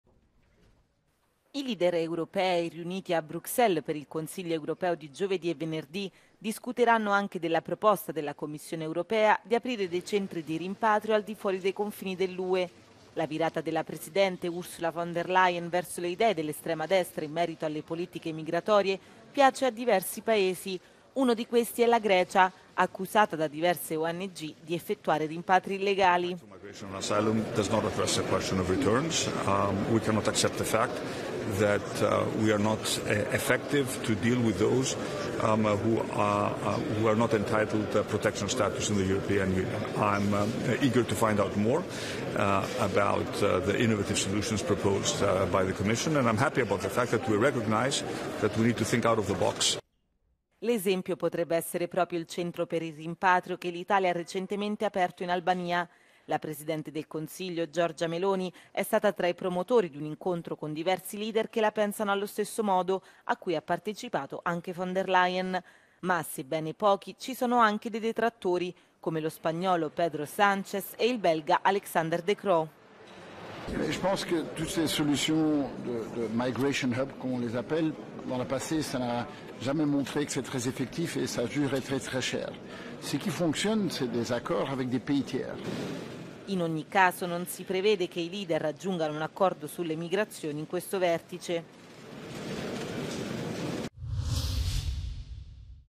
Consiglio europeo, sul tavolo gli hub di rimpatrio fuori dall'Ue proposti dalla Commissione
La presidente della Commissione europea Ursula von der Leyen è sempre più vicina alle idee dell'estrema destra sulle politiche migratorie e vorrebbe replicare in chiave europea il protocollo dell'Italia con l'Albania per esternalizzare la gestione dei migranti. Molti Paesi membri sarebbero d'accordo